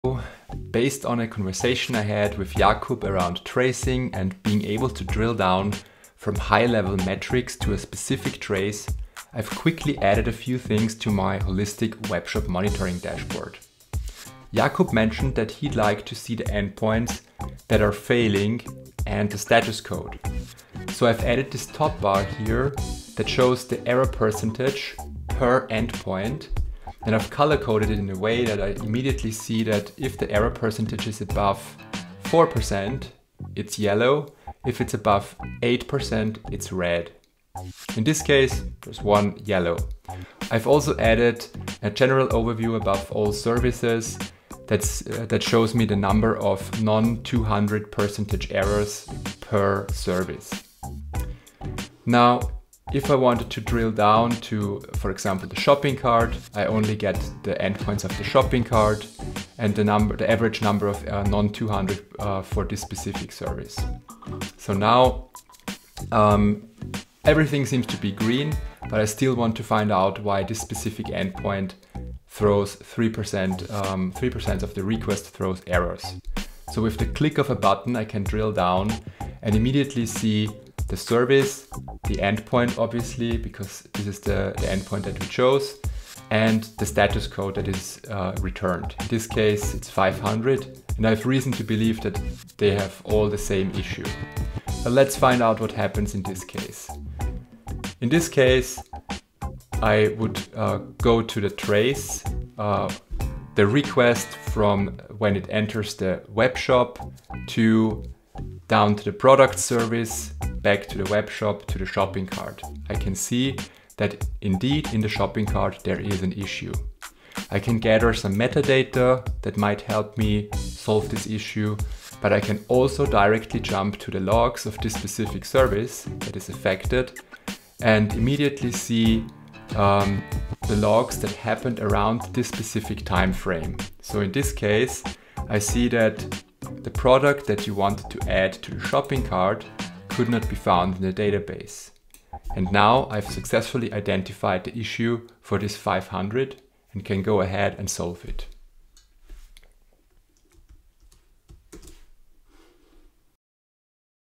Use the Grafana Stack to Drill Down from High Level KPIs to the Underlying Issue
In this short demo video, you'll see how you go from a very general technical overview to quickly finding a problem in your micro services architecture based applications.
With a single click you jump into the relevant endpoint trace, gather the corresponding meta data and with another click nail down the log line that tells you why your application is failing.

Metrics, logs and traces are stored in Prometheus, Loki and Tempo. Instrumentation is done using OpenTelemetry.